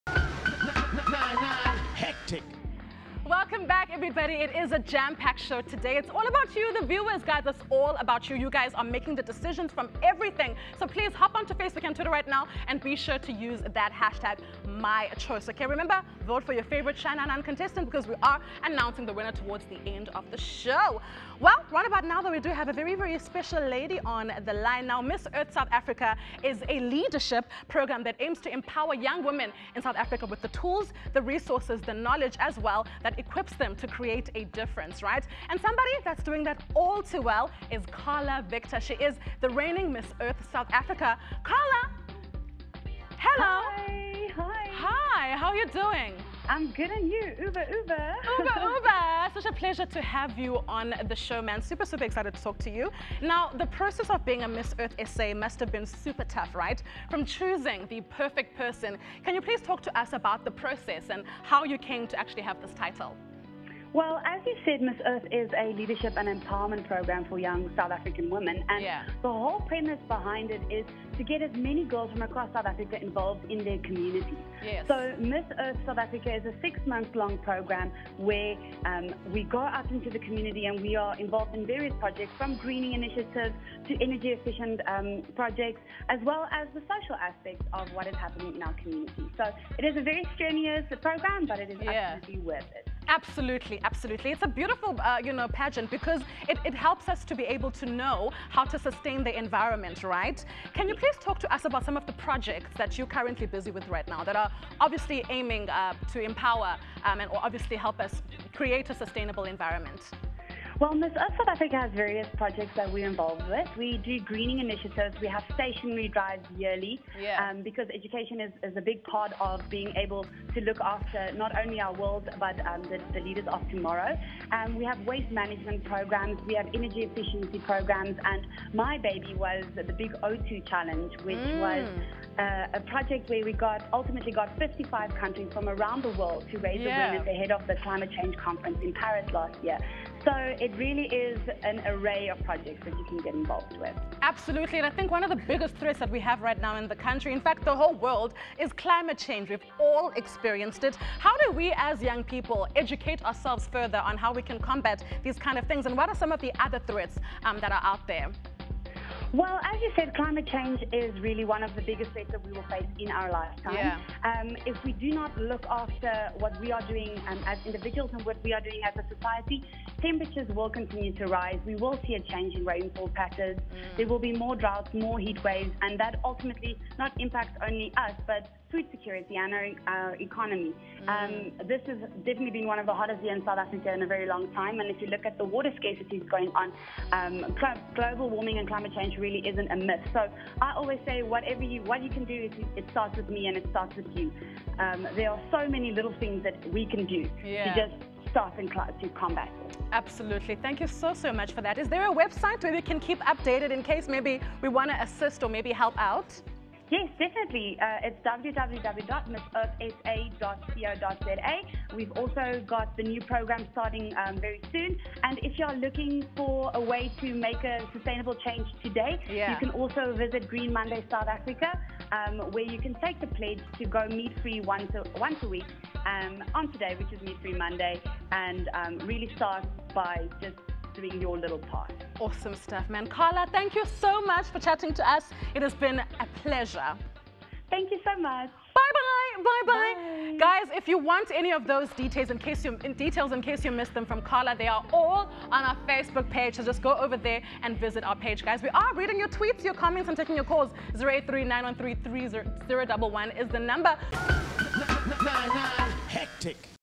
Carla Victor Interview - Celebs on HN9
Watch as we find out more about how to help the environment with @missearth_sa's @carlaviktor!